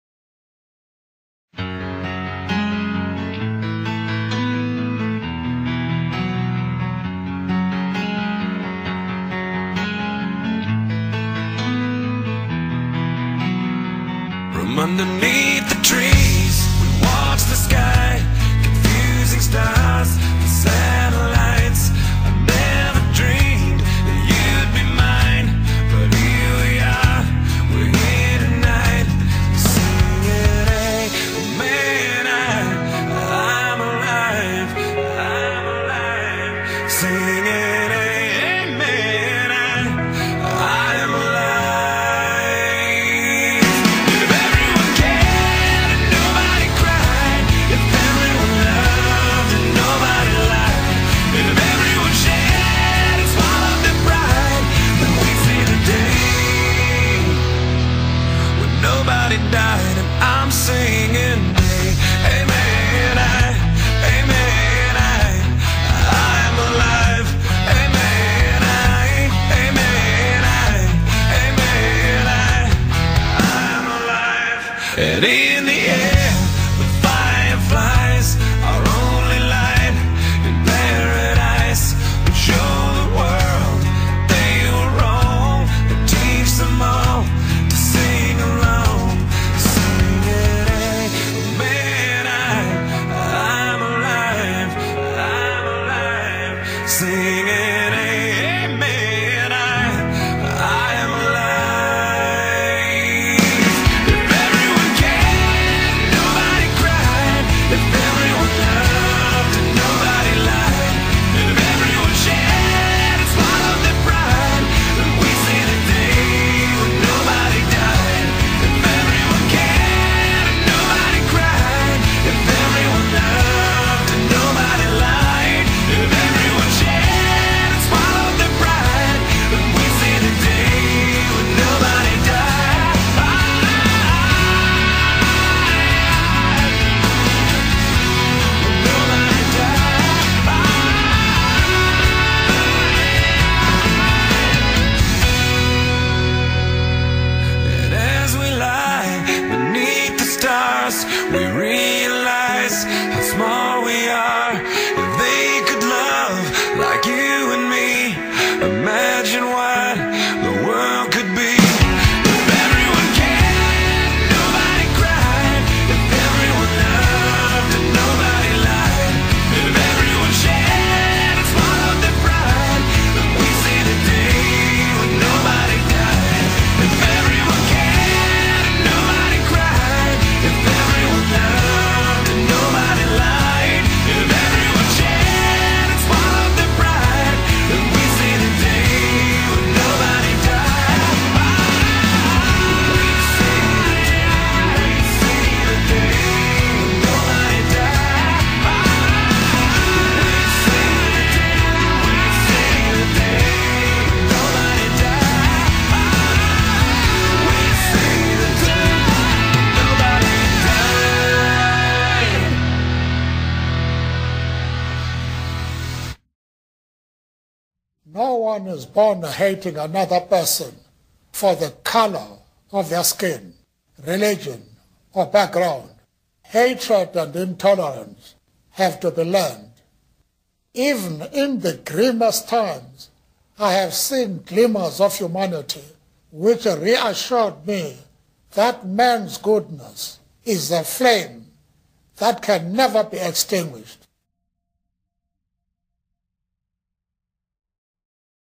Nelson Mandela- A Hero
A project for one of my classes. We had to make a photo story on someone we believed to be a hero. I chose this man because well. watch this and you'll know why.